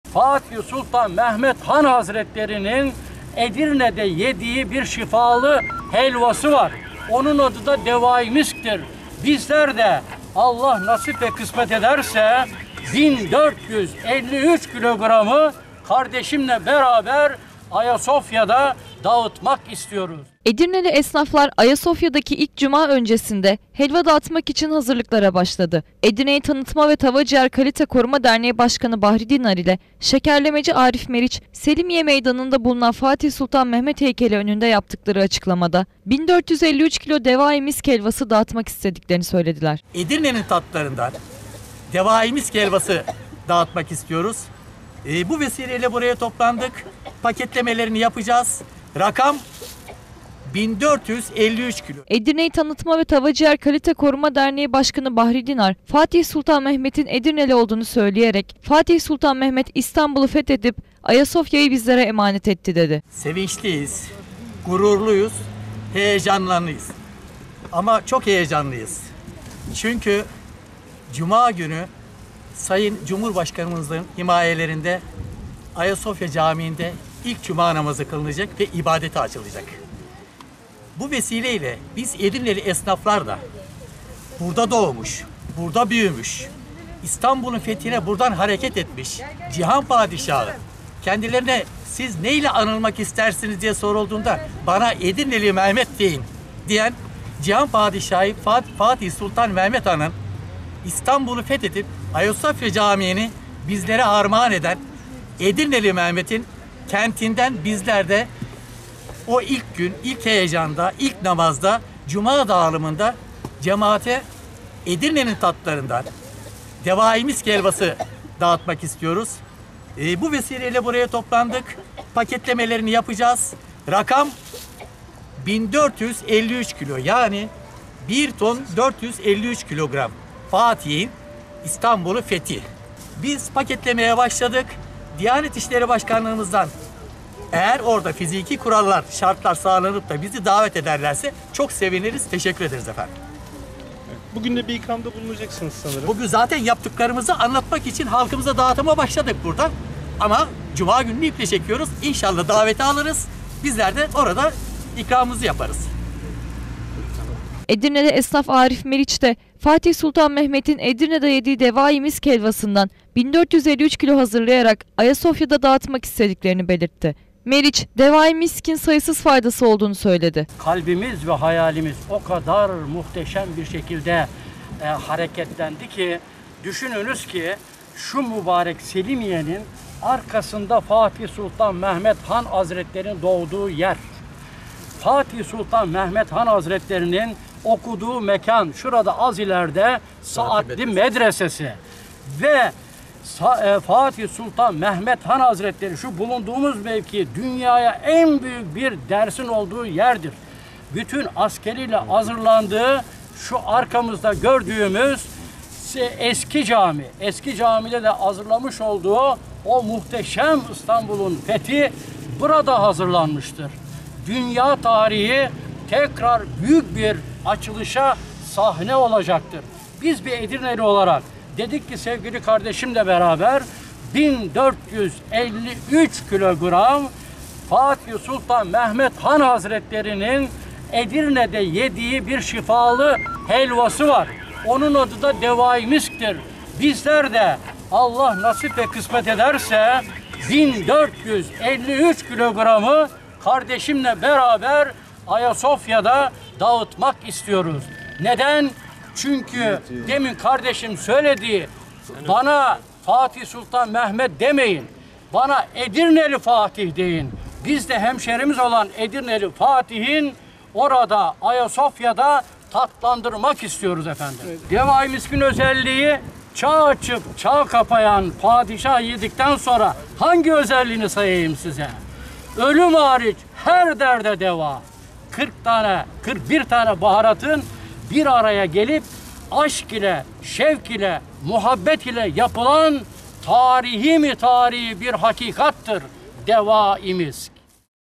AYASOFYA'DA 1453 KİLO DEVA İ MİSK DAĞITACAKLAR
Edirneli esnaflar, Ayasofya’da kılınacak ilk Cuma namazı öncesinde 1453 kilo helva dağıtmak için hazırlıklara başladı. Edirne’yi Tanıtma ve Tava Ciğer Kalite Koruma Derneği Başkanı Bahri Dinar, Fatih Sultan Mehmet’in Edirneli olduğuna dikkat çekerek, Ayasofya’da kılınacak ilk Cuma namazı sonrasında Devai misk helvası dağıtmak istediklerini söyledi.